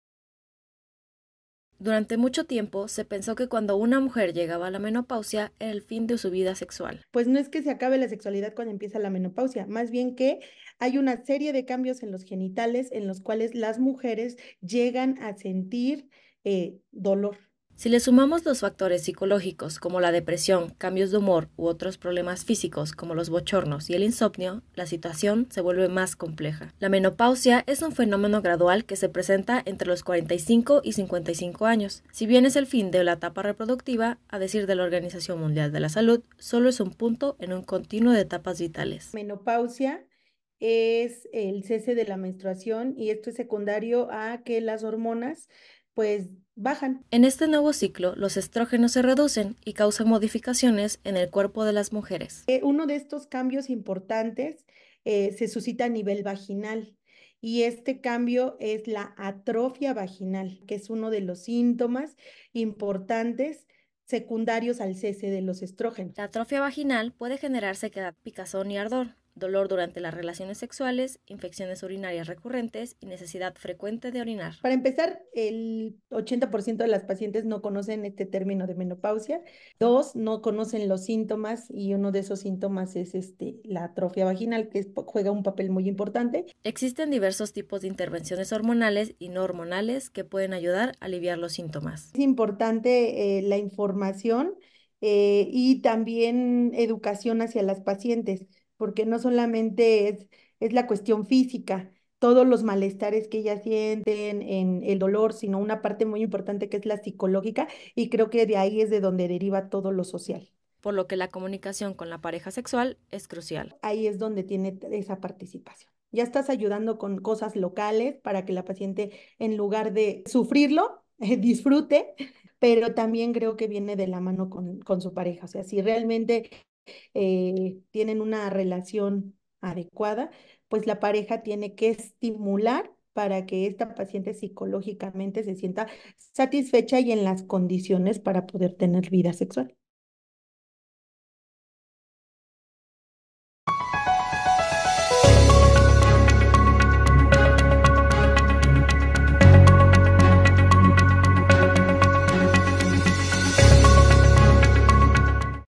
SEXUALIDAD EN LA MENOPAUSIA: ¿CÓMO DISFRUTAR DE LAS RELACIONES?
DURANTE MUCHO TIEMPO SE PENSÓ QUE CUANDO UNA MUJER LLEGABA A LA MENOPAUSIA, ERA EL FIN DE SU VIDA SEXUAL…